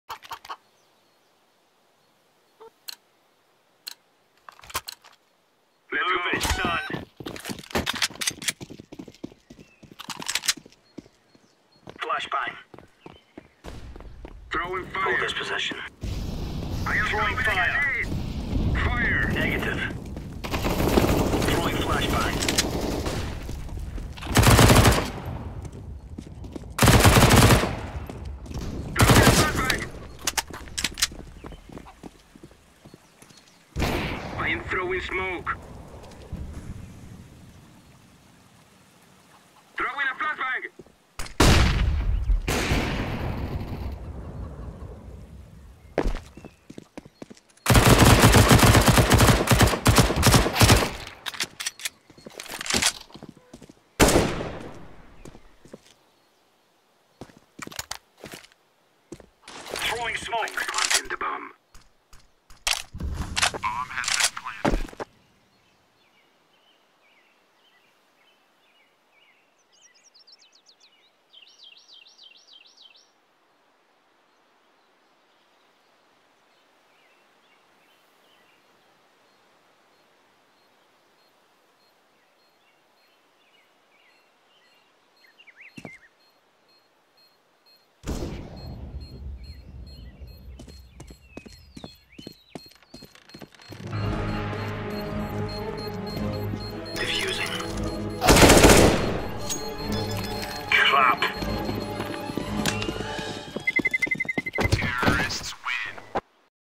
QUE TIRO FOI ESSE V14D00000 ?!?!
NEM EU ENTENDI OQ EU FIZ, MAS MATEI E AJUDEI O TIME ENTAO É ISSO QUE VALE.